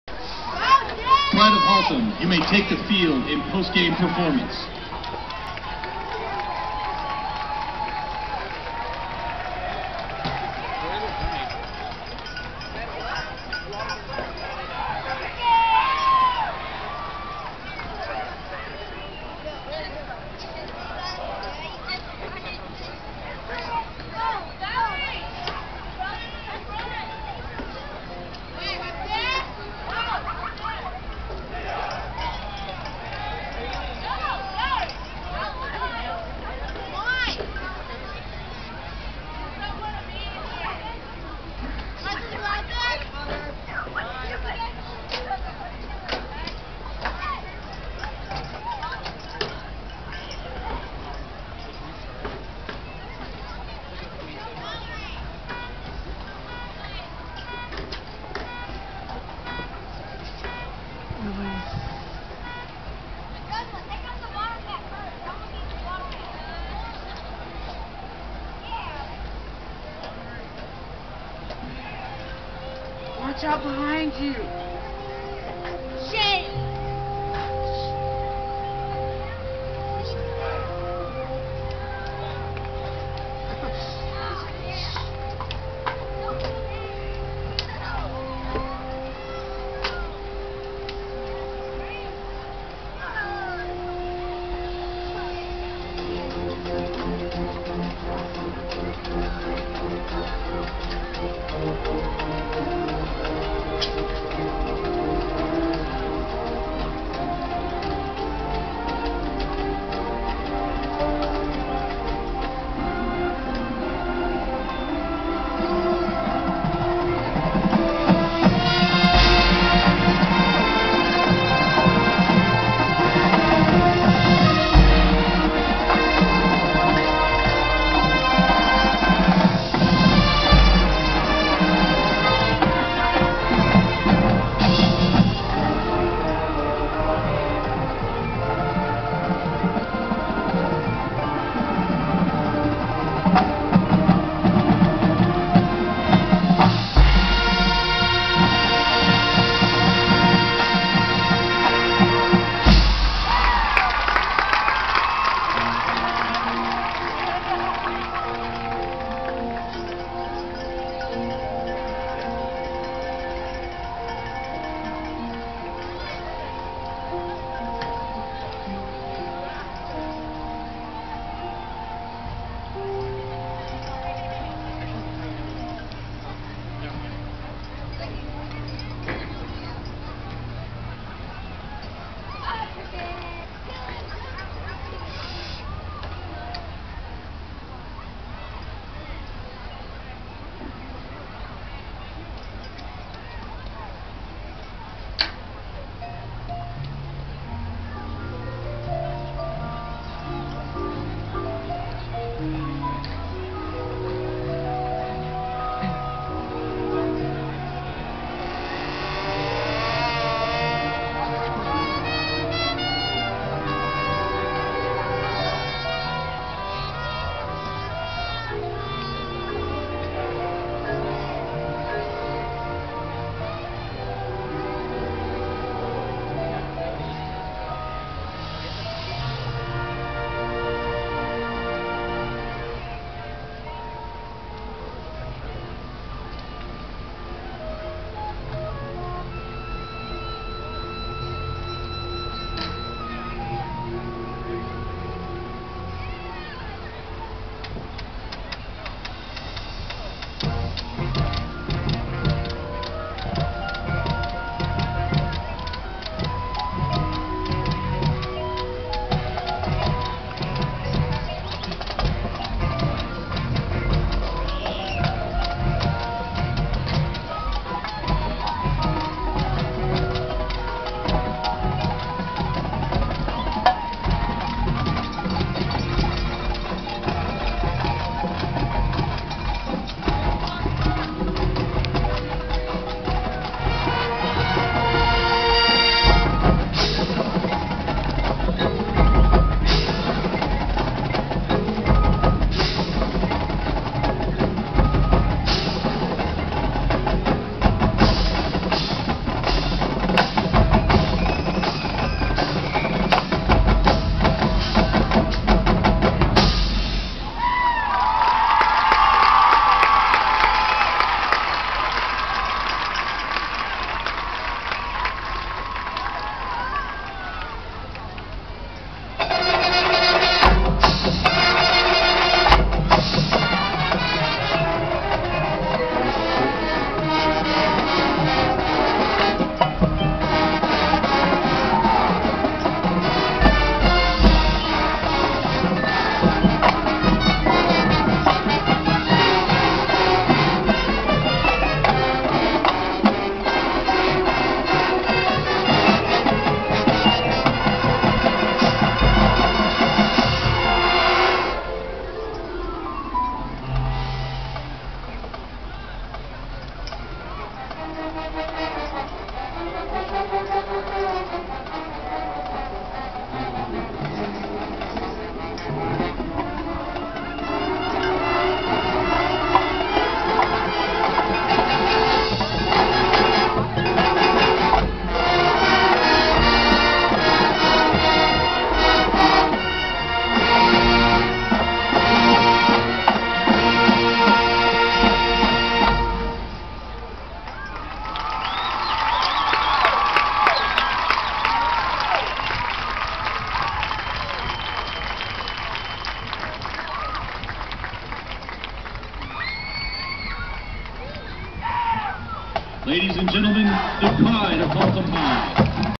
Haltom High School 2009 Production (homecoming)
This is haltom's early show for 2009 at the keller game for homecoming sry bout the dam car alarm and stupid kids they wouldn't stop talking enjoy rate comment